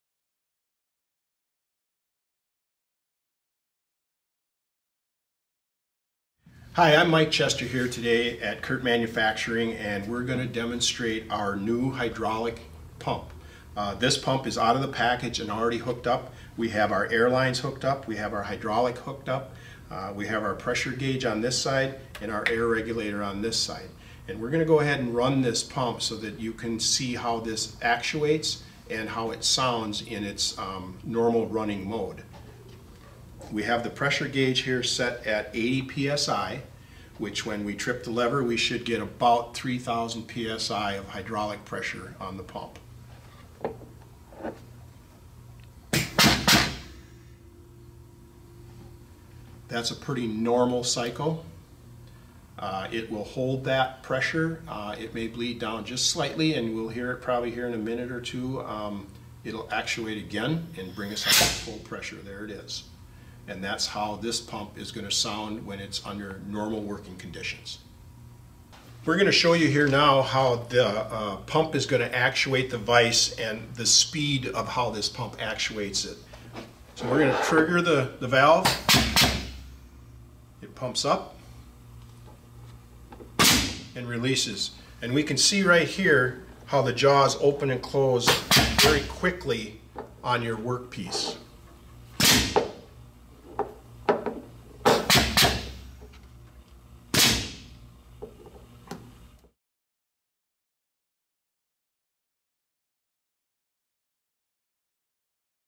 Kurt KHP3 Hydraulic Pump - Pump Demonstration
Kurt KHP3 pumps, our best-selling air-over-hydraulic models, operate with shop air and can be configured as single- or double-acting and run from one vise or multiple vises on a single pump. Pumps operate semi-automated with an operator controlling the pump using a foot switch (KHP3F), hand switch (KHP3H) or a toggle (KHP3T). KHP3 pumps can also be configured to run fully automated using a machine tool controller, robot or PLC.